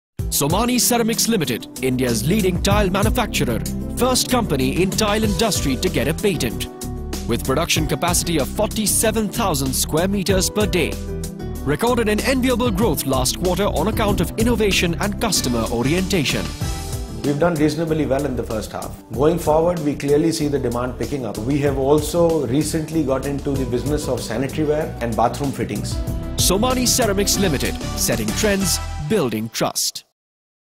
SOMANY CERAMICS Q2 2009 10 RESULTS ON CNBC
Somany Ceramics announced its Q2 results for the year 2009-10. Despite of slowdown in the market, it has sustained its growth.